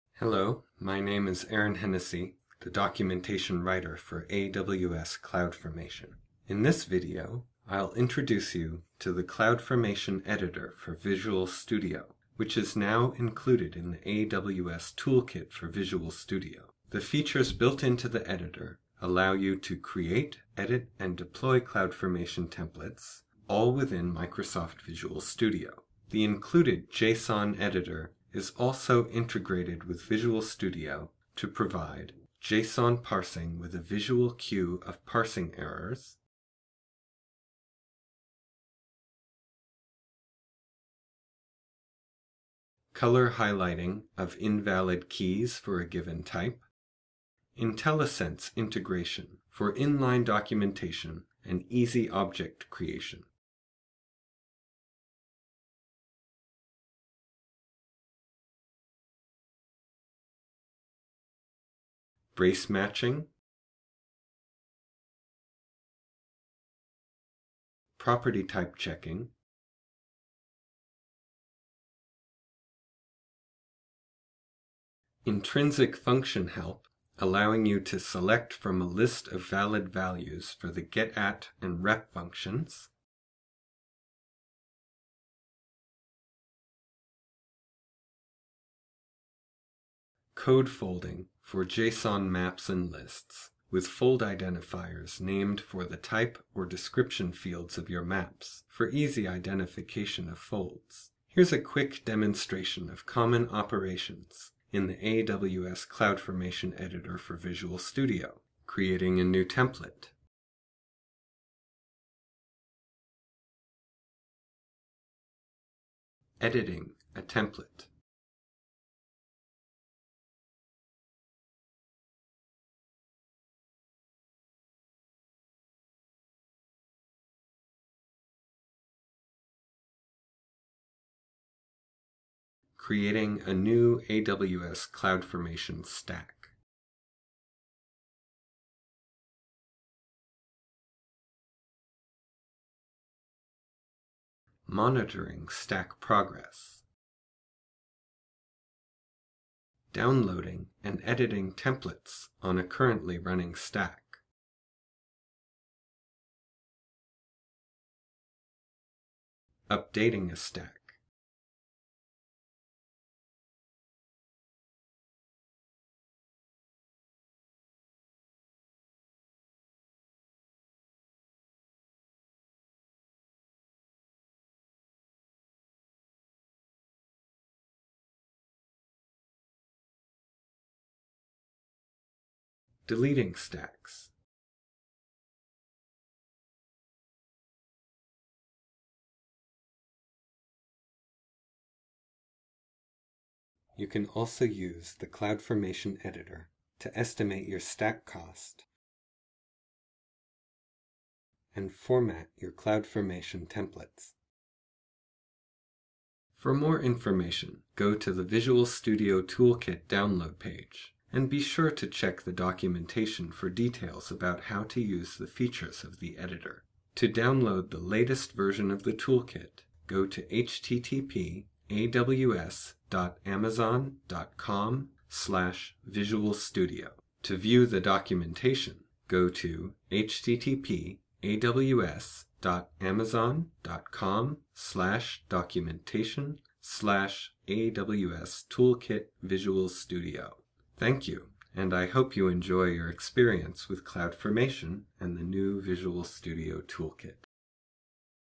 Introduction to the AWS CloudFormation Editor for Visual Studio
This video provides a demonstration of many of the features included in the CloudFormation template editor for Visual Studio, part of the AWS Toolkit for Visual Studio.